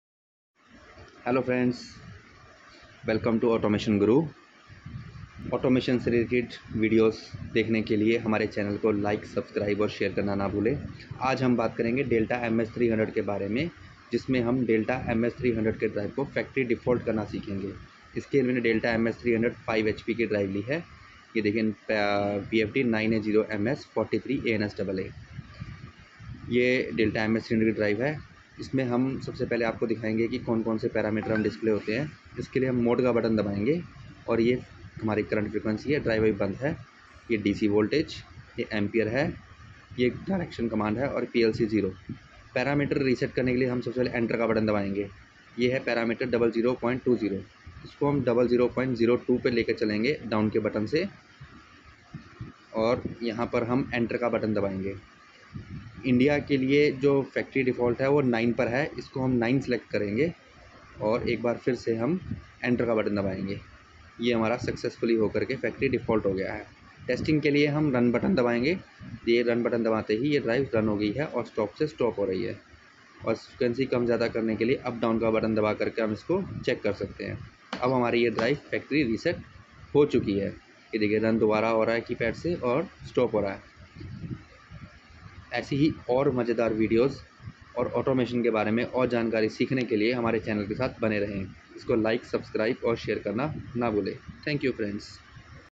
DELTA MS300 PARAMETER FACTORY DEFAULT/RESET
LEARN AUTOMATION WITH AUTOMATION GURU!!
Learn how to make DELTA MS300 to Factory Default .
Parameter reset to factory Default.